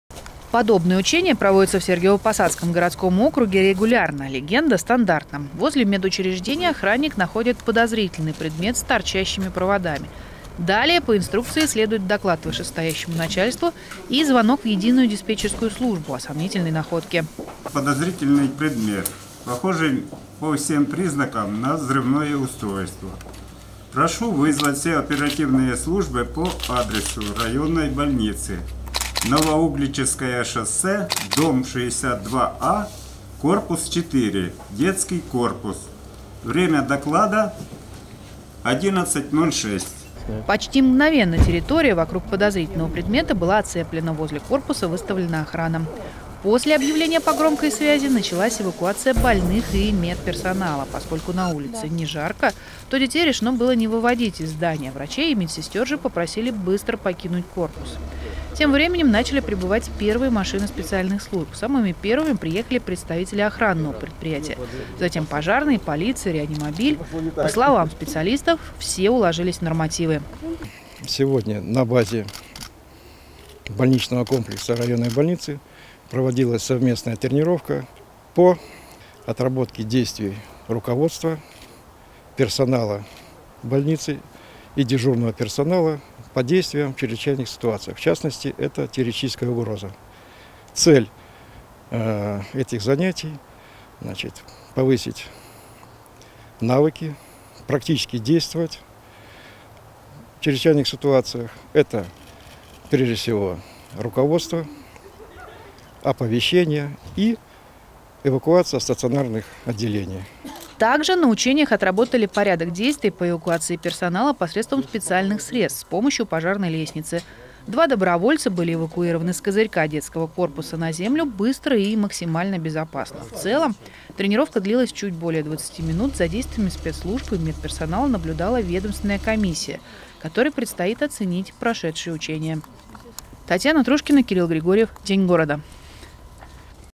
Антитеррористические учения в ЦРБ
В эту среду на территории Сергиево-Посадской районной больницы в детском корпусе прошли антитеррористические учения. По легенде, рядом с медучреждением был обнаружен подозрительный предмет с проводами. Во время тренировки было отработано взаимодействие специальных служб и персонала больницы.